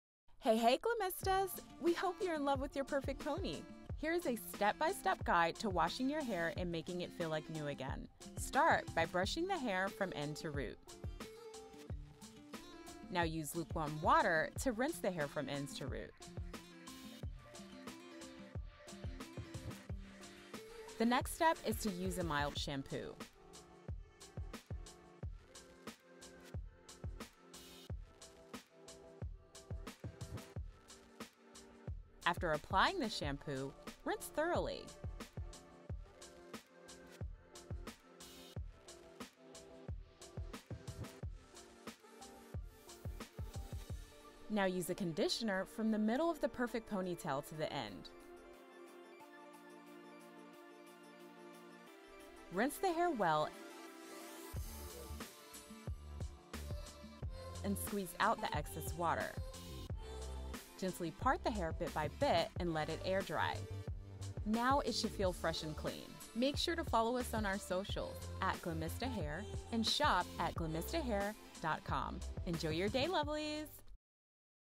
HOW TO WASH THE PERFECT PONY
Step by step guide to wash your synthetic ponytail.

Our perfect ponytails are super easy to wash and they will feel like new again.